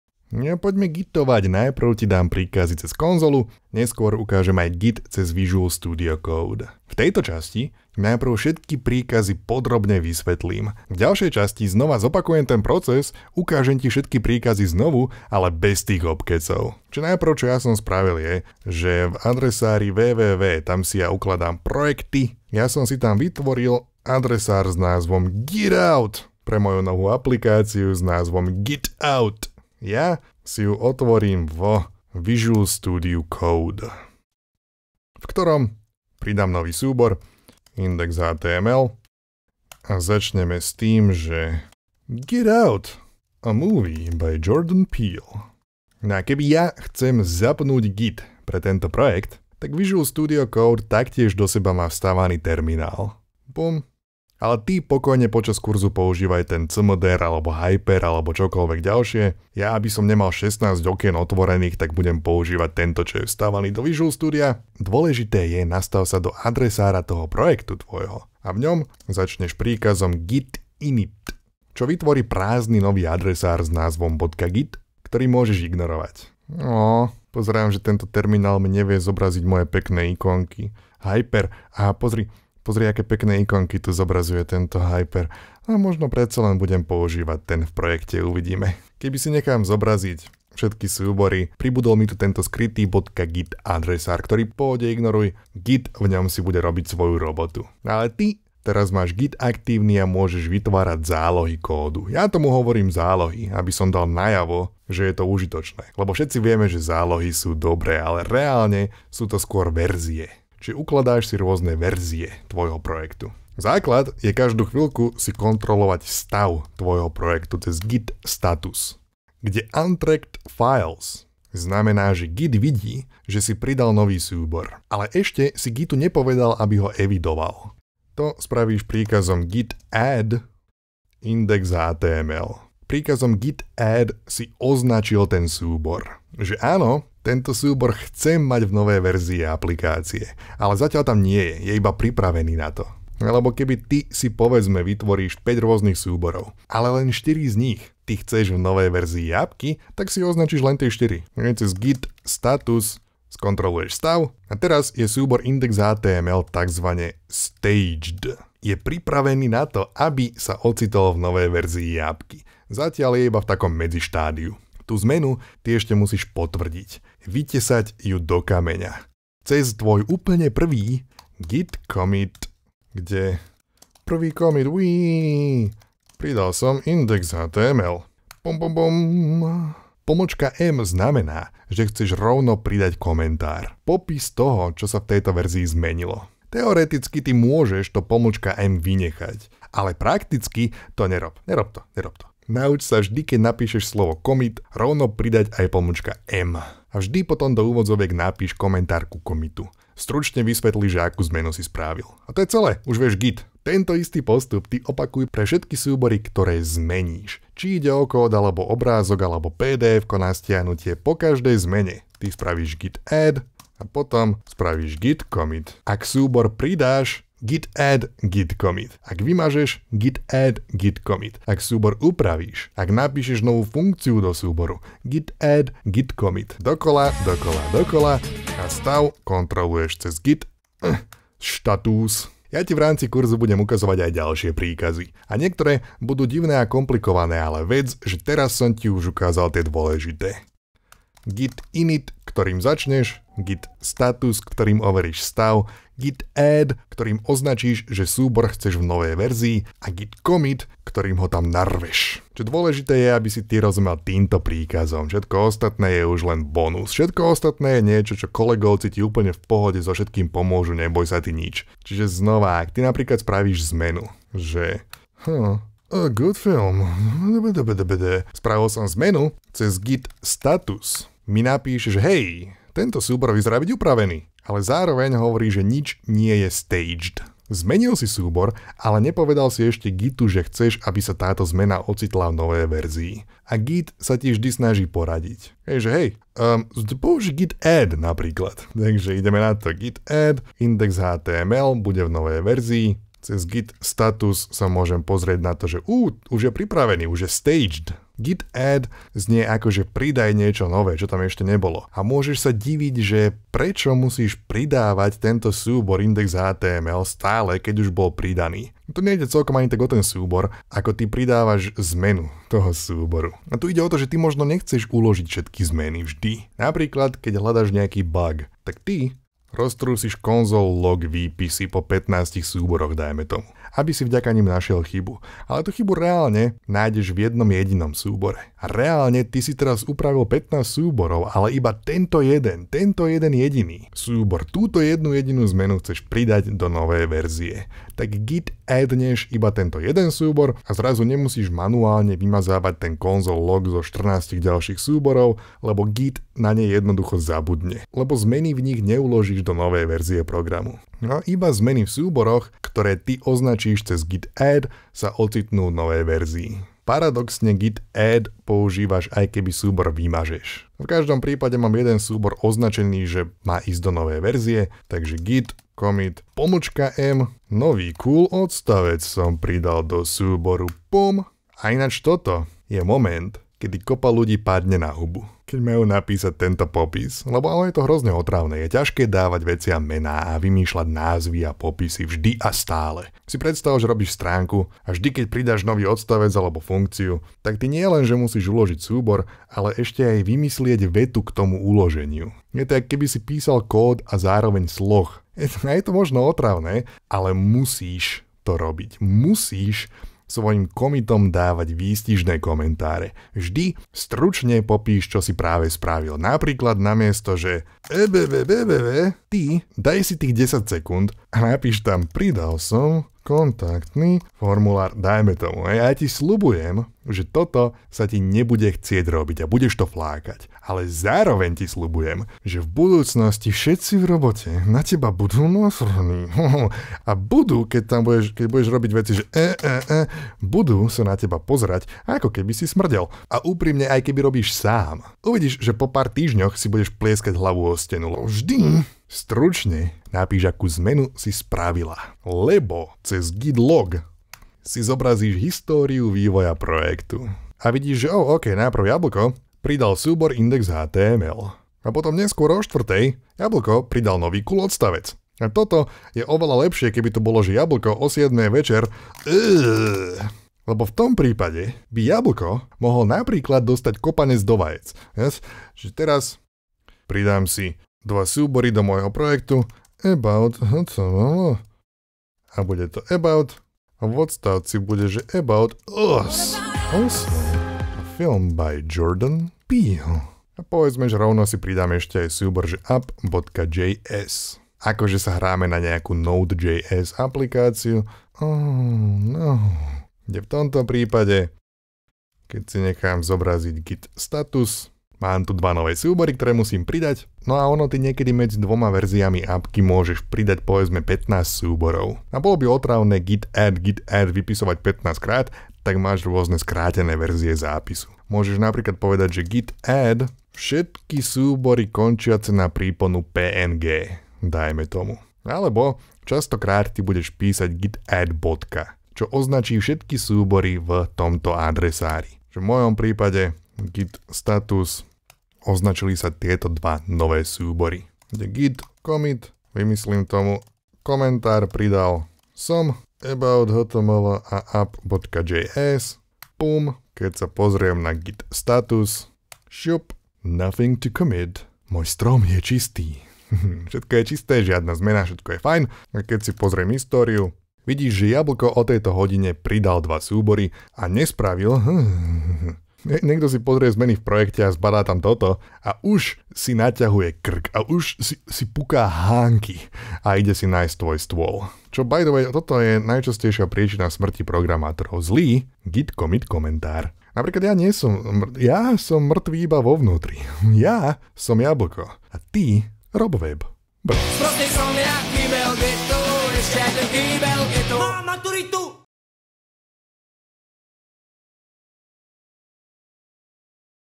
Naučím ťa Git príkazy 🇸🇰 Git a GitHub kurz (4/15)
Dnes dám zásadné Git príkazy! A taktiež videá a kurzy skôr! Yay! Poď gitiť jak drak.

00:00 Pripravím si KÓD
01:04 GIT INIT (zapni git pre projekt)
01:53 GIT STATUS (over stav projektu)
02:07 GIT ADD (označ súbor)
02:44 GIT COMMIT (nový zárez do histórie)
03:23 Rekapitulácia
04:21 Prečo musíš GIT ADD volať dokola?
06:00 PRIDÁVAJ 👏 COMMITOM 👏 KOMENTY 👏
07:22 GIT LOG (zobraz históriu projektu)
07:45 GIT ADD (označ viac súborov naraz)

naučím ťa programovať